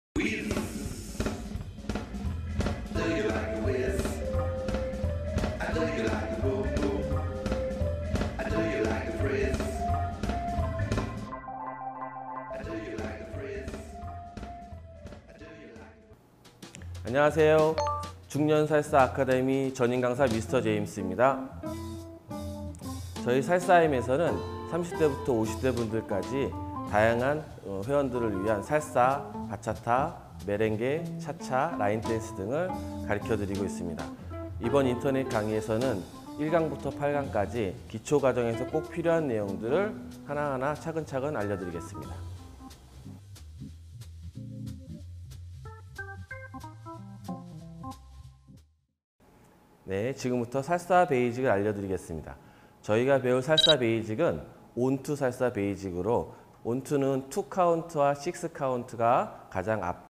[미스터제임스의 살사 댄스 배우기 (기초편)] 살사댄스 1강
15년 경력의 살사댄스 인스트럭터 미스터제임스의 노하우를 알려드립니다.
처음 살사를 하는 분을 위한 수업내용이기도 하지만,
처음 배우시는 분들보다 한번 배웠던분들,
그리고 이미 배워서 추고 계신 분들에게도 꼭 필요한 내용의 수업입니다.
어떻게 하면 더 잘 춤을 출수 있을까에 대한 고민을 해결하기 위한 깊이 있는 내용을 포함합니다.
궁금한것이 있으면 언제든지 물어보세요!!